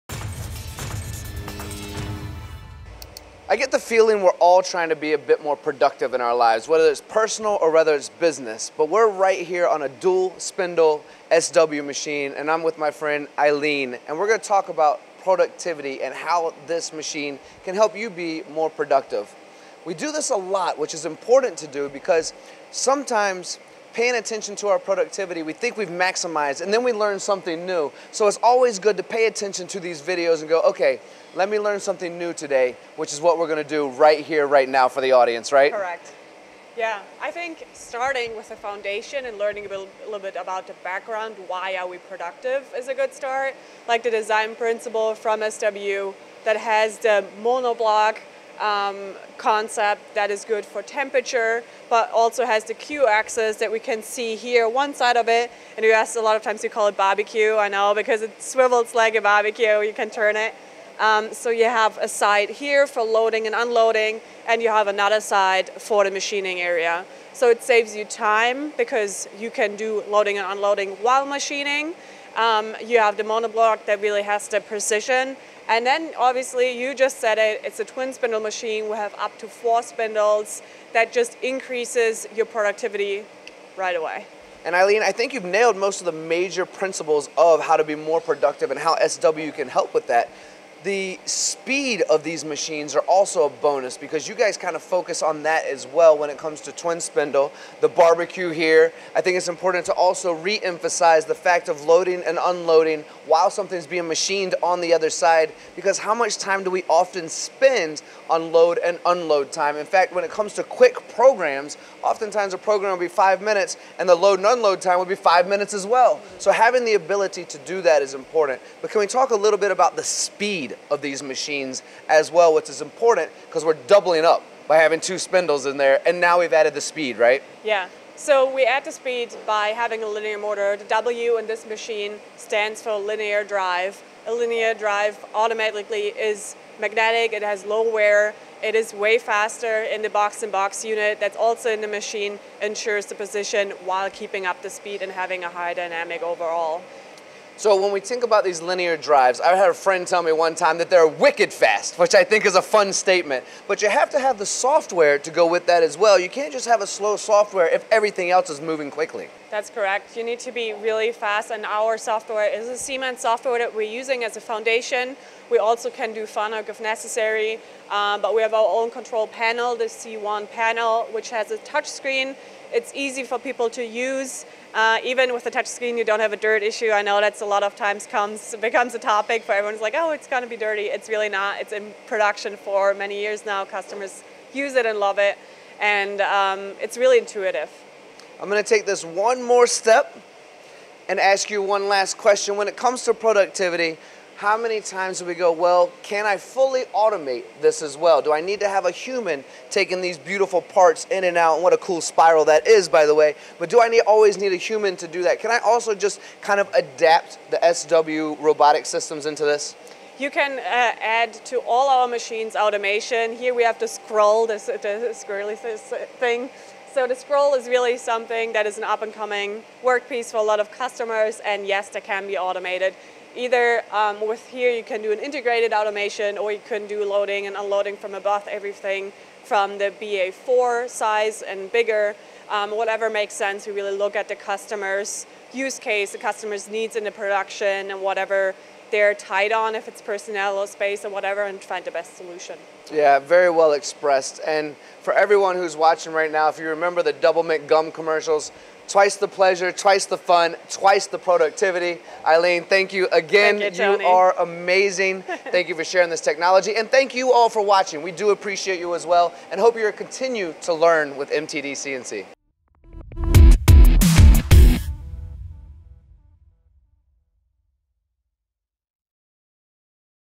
SW machines are designed for increased productivity
Tony Gunn of MTDCNC meets Eileen Fries of SW to understand how SW machines can help customers become more productive. Standing in front of a dual-spindle machine, Eileen shares the SW design principle that has the monoblock concept for precision and queue access for loading and unloading while machining. Combined with the speed of a linear motor, fast SIEMENS software, and an easy-to-use C1 touchscreen control panel, this machine is built for maximised productivity. Automation can be added to all SW machines, but SW look at customers’ needs and provide the best solution for each use case.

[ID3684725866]